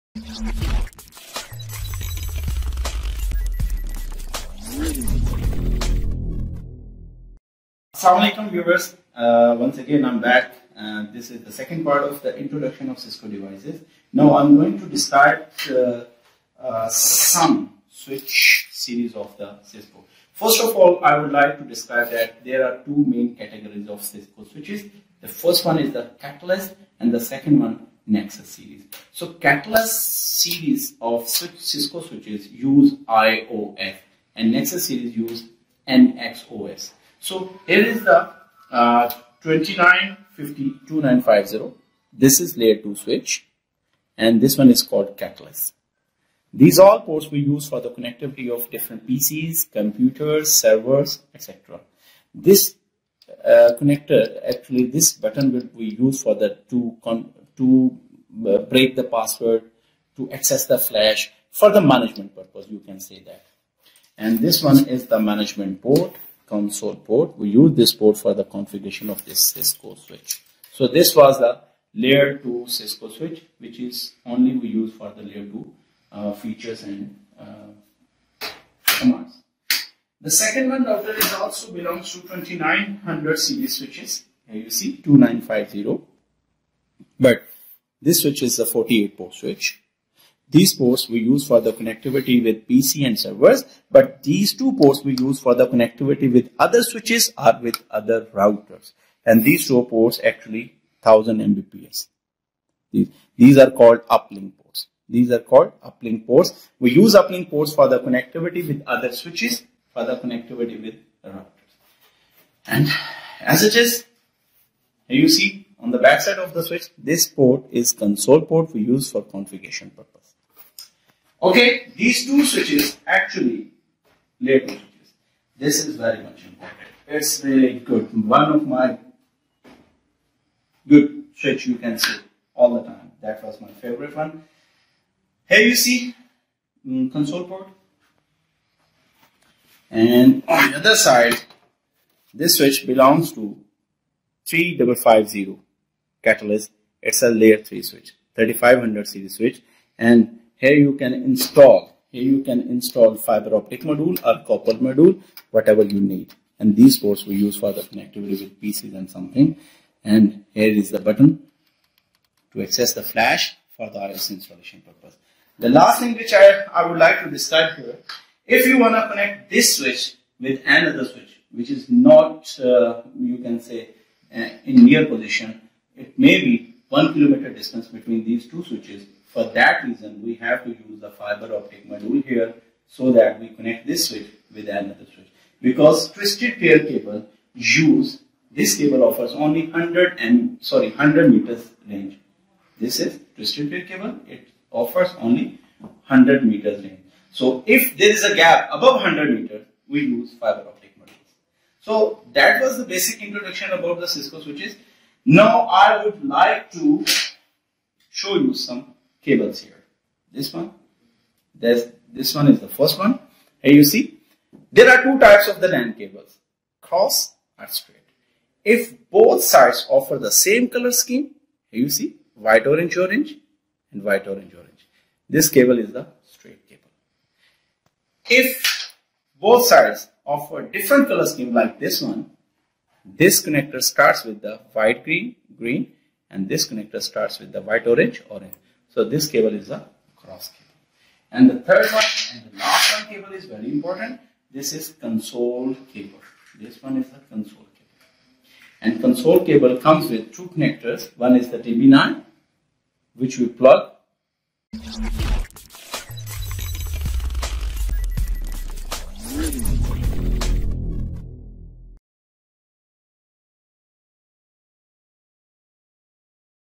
Identification of Switch Series (English)
Hello Welcome to CCNA Routing and Switching Training Series.
In this lecture you can learn about Switches and its series.There are two categories of Cisco Switches.Catalyst Series and nexus series.Catalyst Series contain Ios and nexus series contain nxos.
There are different series of Cisco Switches. One series is 2500 and other is 3500 series.Series are different according to their hardware and software. 2500 Series called layer 2 Switch and 3500 series called Layer 3 Switch. L2 switch understand MAC Address and L3 Switch understand MAC Address and IP address. We can insert different modules in switch.
We use cross cable to connect switch with other switch and we use console cable to connect switch with computer.